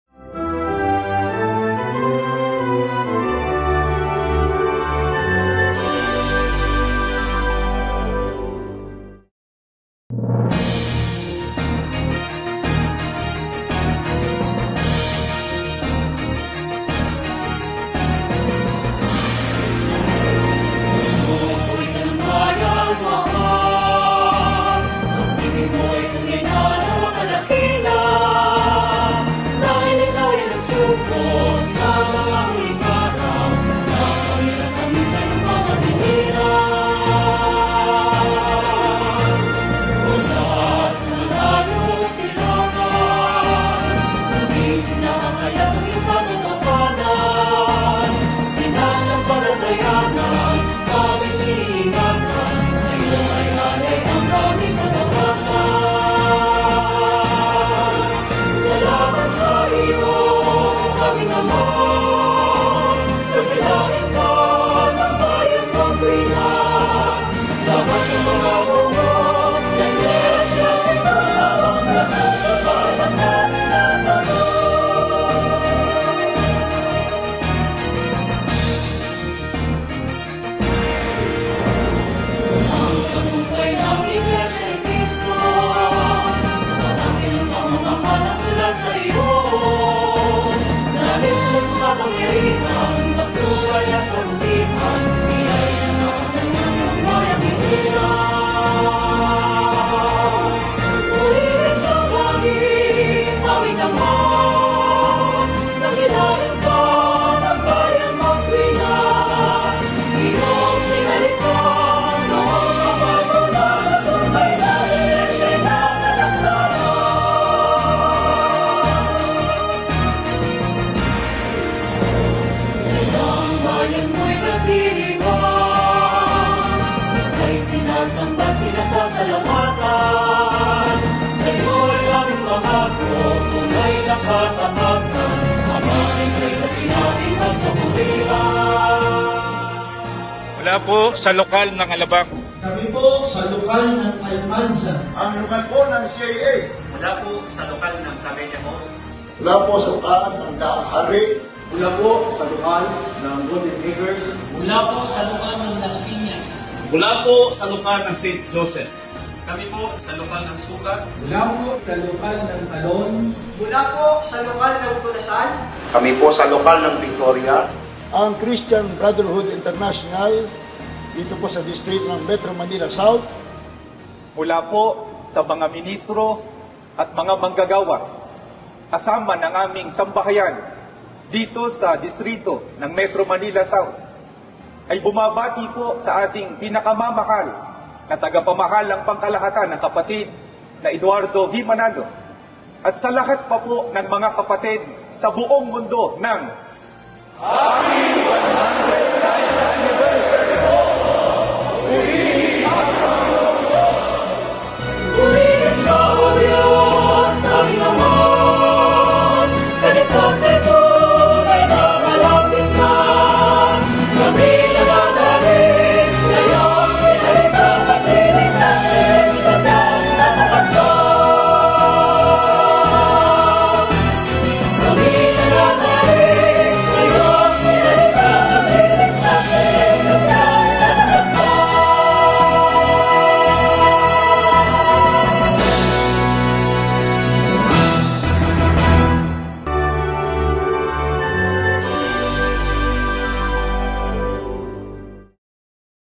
Metro Manila South | Pagbati para sa Ika-109 Anibersaryo ng Iglesia Ni Cristo
Pagbati mula sa Distrito Eklesiastiko ng Metro Manila South para sa Ika-109 Anibersaryo ng Iglesia Ni Cristo